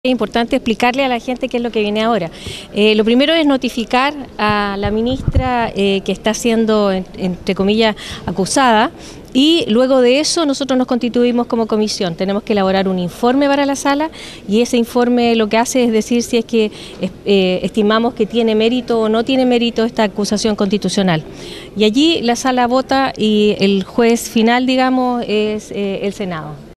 Diputada Hernando: "Veremos si hay o no mérito para la acusación constitucional"
La parlamentaria explicó que una vez presentada la acusación constitucional, se le notifica a la Ministra de Salud, y luego se elabora un informe por parte de una comisión parlamentaria, que luego debe ser votado en las salas de la Cámara y el Senado, donde finalmente se decide si procede o no acusar constitucionalmente a la Ministra de Salud, Carmen Castillo.